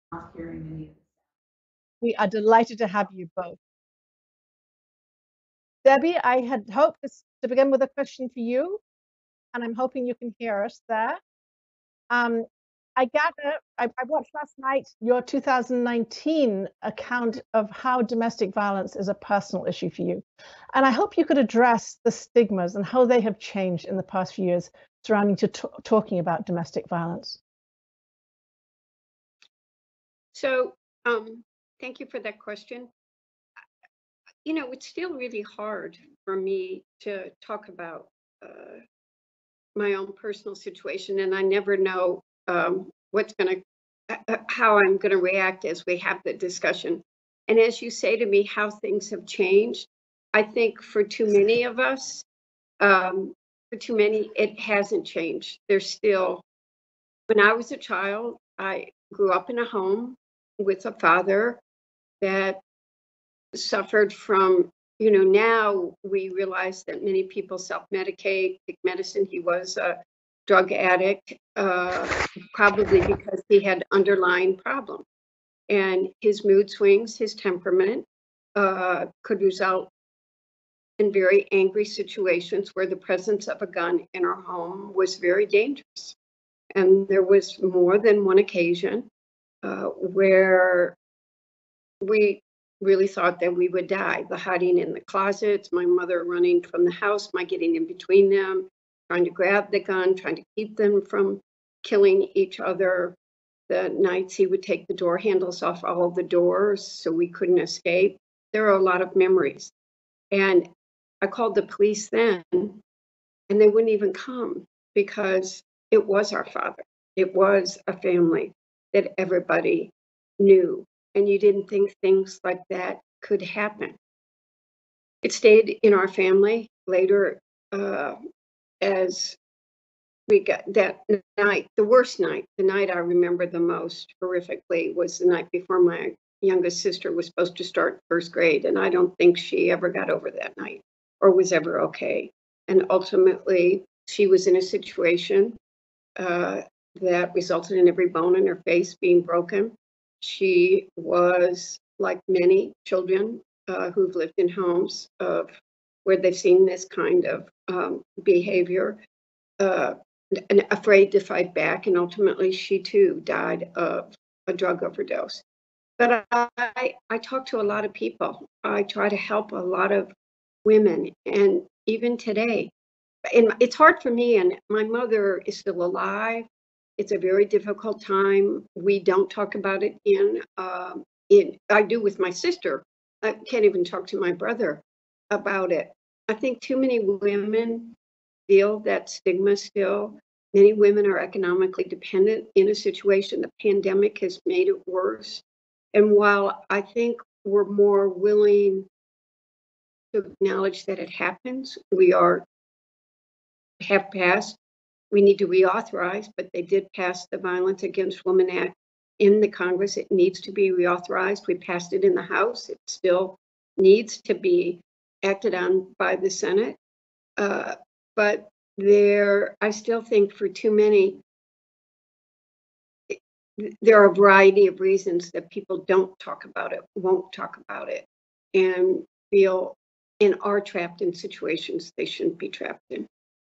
Rep. Debbie Dingell recounts horrific childhood experience with domestic violence:
“When I was a child, I grew up in a home with a father that suffered from… he was a drug addict, probably because he had underlying problems. And his mood swings, his temperament could result in very angry situations where the presence of a gun in our home was very dangerous. And there was more than one occasion where we really thought that we would die. The hiding in the closet, my mother running from the house, my getting in between them, trying to grab the gun, trying to keep them from killing each other. The nights he would take the door handles off all the doors so we couldn’t escape. There are a lot of memories. And I called the police then, and they wouldn’t even come because it was our father, it was a family that everybody knew and you didn’t think things like that could happen.” – Rep. Debbie Dingell (D-Mich)